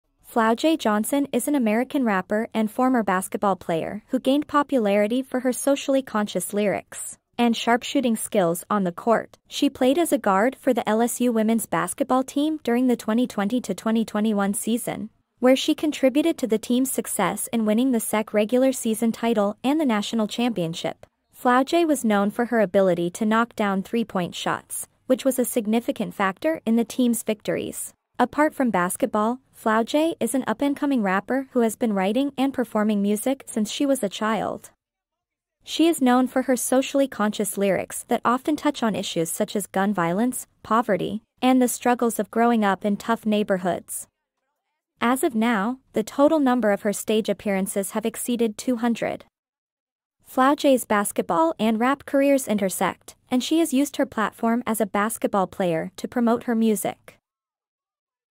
Flau'jae: LSU's Sharpshooting Guard: A Rising Star in Women's Sports & Socially Conscious Rapper
Welcome to our women's sports channel! Today, we are excited to bring you a feature on the multi-talented Flau'jae Johnson. Flau'jae is an American rapper and former basketball player who has been making waves in both fields.

As a guard for the LSU Women's Basketball team during the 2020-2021 season, Flau'jae contributed significantly to the team's success in winning the SEC regular season title and the national championship. She was known for her sharpshooting skills on the court, particularly her ability to knock down three-point shots.

Flau'jae's skills on the court are just a small part of what makes her an impressive athlete. In addition to basketball, she has been writing and performing music since she was a child. Her socially conscious lyrics have gained her a following in the rap community, touching on issues such as gun violence, poverty, and growing up in tough neighborhoods. As of now, Flau'jae has performed on stage over 200 times, cementing her status as a rising star in the music world.

Flau'jae's basketball and rap careers intersect in interesting ways. She has used her platform as a basketball player to promote her music, performing at halftime shows during LSU basketball games. Her experiences as a basketball player have informed her music, drawing on themes such as perseverance, teamwork, and dedication.

We hope you enjoyed learning more about Flau'jae Johnson's remarkable talent in both basketball and music.